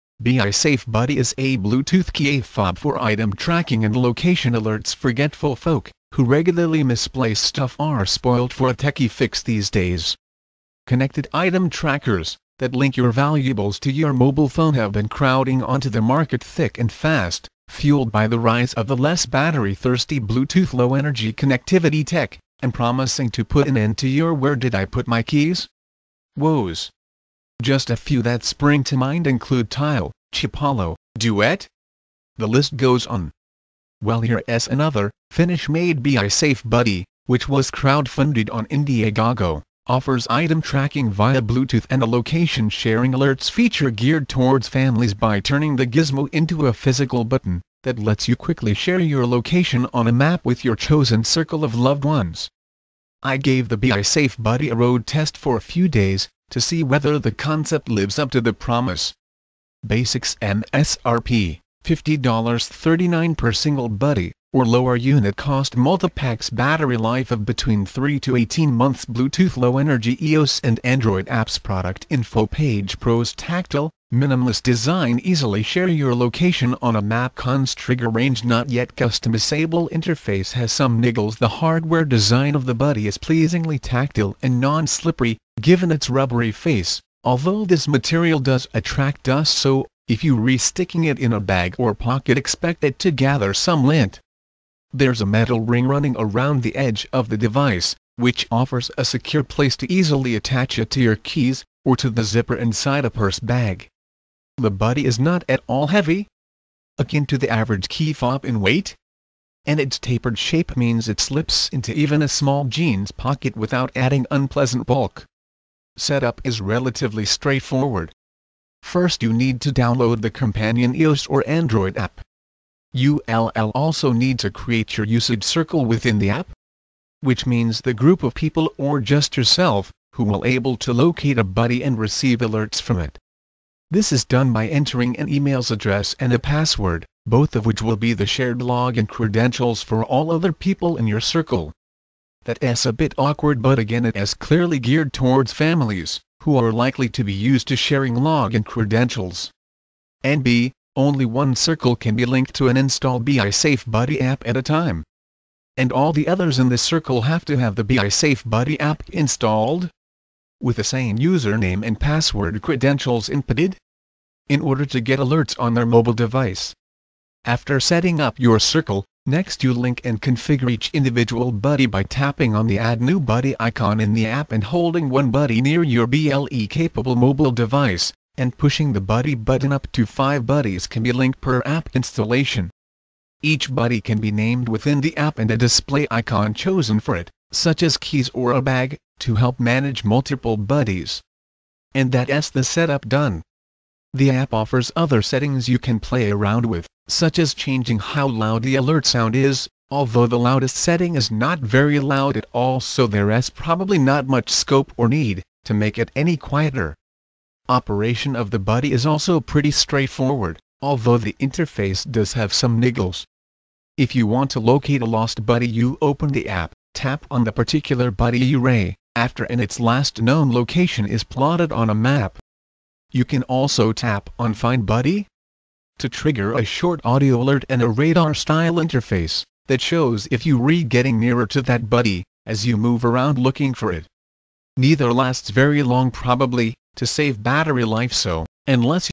BiiSafe Buddy Is A Bluetooth Keyfob For Item-Tracking And Location Alerts
BiiSafe Buddy Is A Bluetooth Keyfob For Item-Tracking And Location Alerts Forgetful folk who regularly misplace stuff are spoilt for a techie fix these days. Connected item trackers that link your valuables to your mobile phone have been crowding onto the market thick and fast, fueled by the rise of the less battery-thirsty Bluetooth Low Energy connectivity tech, and promising to put an end to your ‘where did I put my keys?’ woes. Just a few that spring to mind include Tile, Chipolo, Duet… the list goes on.